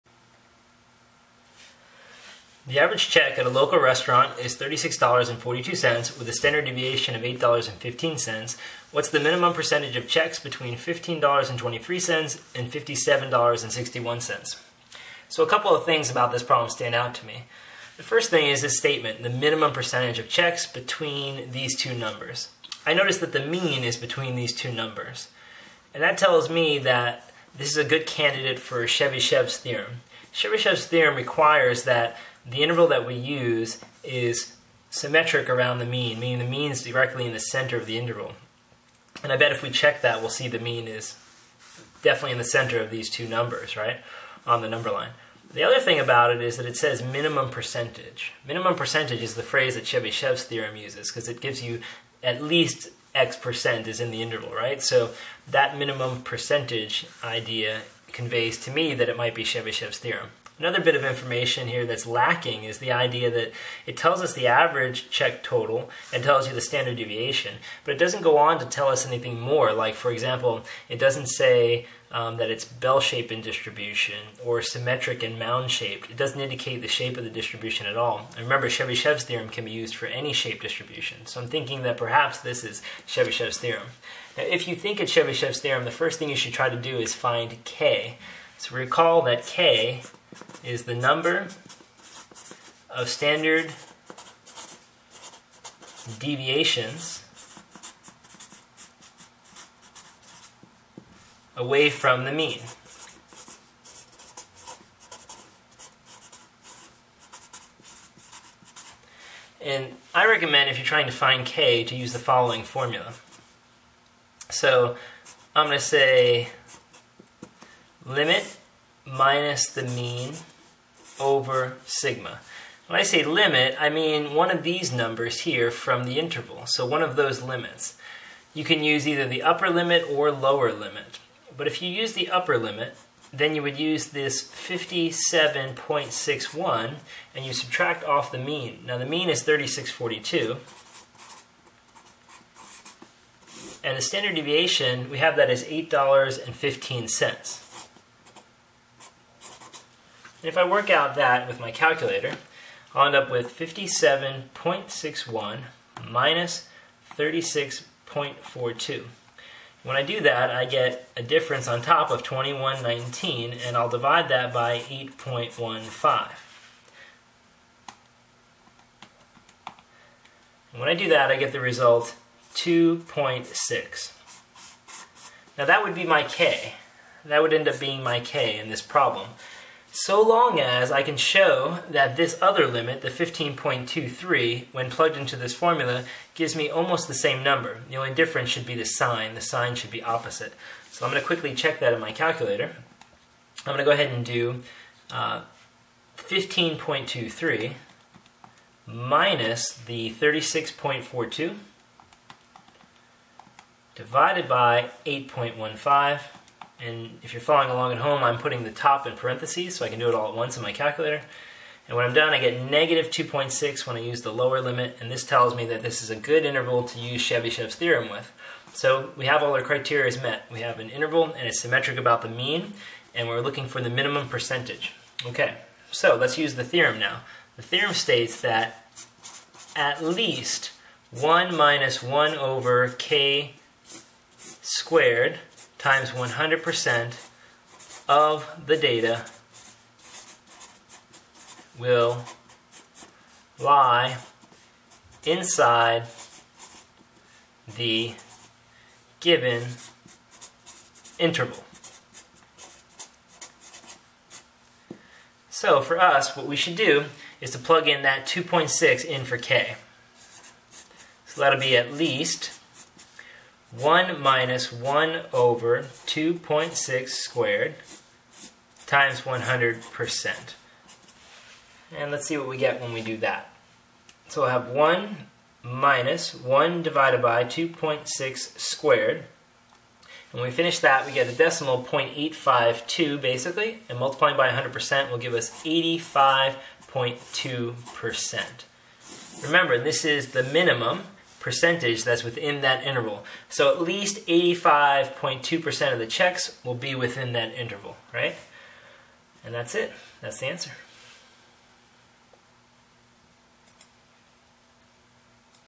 Problem Using Chebyshev's Theorem
This problem is a basic example that demonstrates how and when to apply Chebyshev's Theorem.  organizes all of my YouTube videos in one, easy to navigate place.